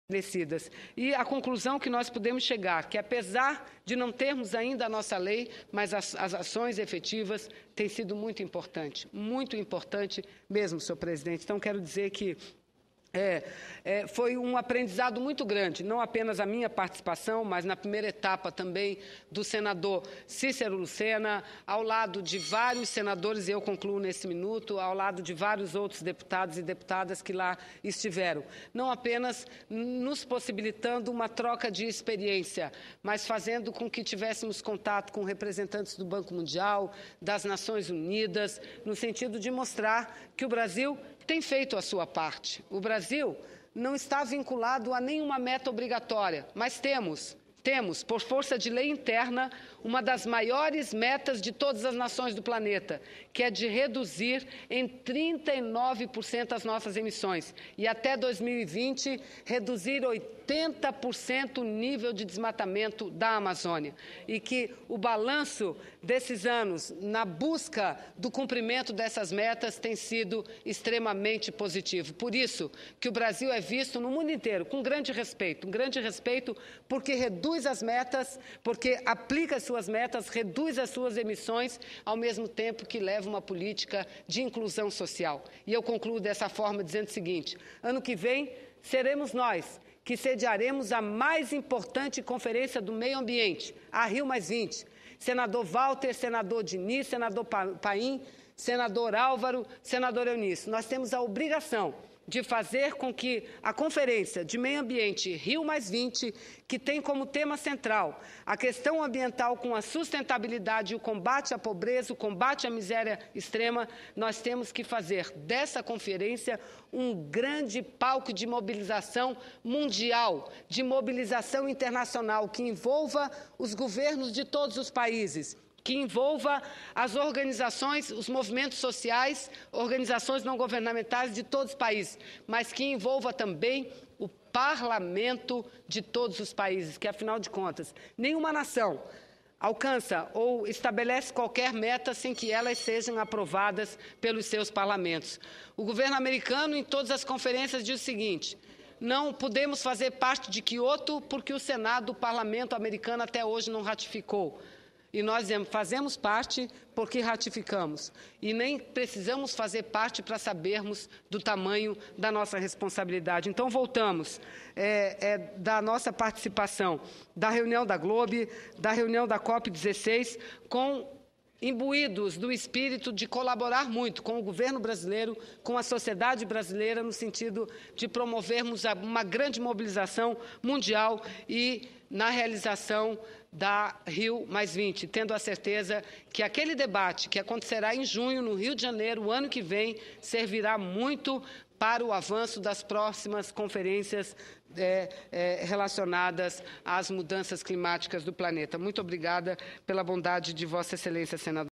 Brasil é visto com respeito por reduzir suas emissões, ressalta Vanessa Grazziotin
12/12/2011 - Plenário - Brasil é visto com respeito por reduzir suas emissões, ressalta Vanessa Grazziotin